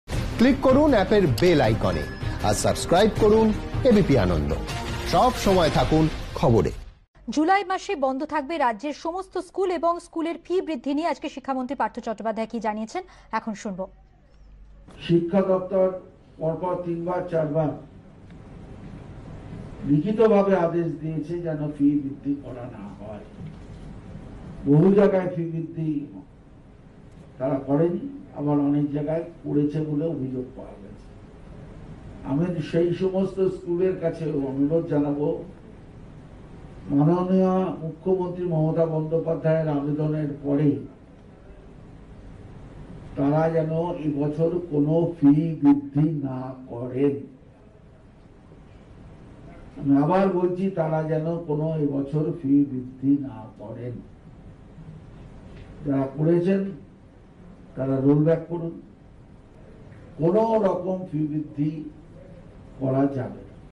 এ বছর স্কুলে ফি বৃদ্ধি করা যাবে না, জানিয়ে দিলেন শিক্ষামন্ত্রী
শিক্ষা দফতর তিন-চারবার লিখিতভাবে আদেশ দিয়েছে, যেন ফি বৃদ্ধি করা না হয়। তারপর অনেক স্কুল ফি বৃদ্ধি করেছে, আবার অনেক স্কুল করেনি। সেই স্কুলগুলির কাছে অনুরোধ জানাব, মুখ্যমন্ত্রী মমতা বন্দ্যোপাধ্যায়ের আবেদনের পর যেন এ বছর ফি বৃদ্ধি না করেন। বর্ধিত ফি প্রত্যাহার করতে হবে, জানালেন শিক্ষামন্ত্রী পার্থ চট্টোপাধ্যায়।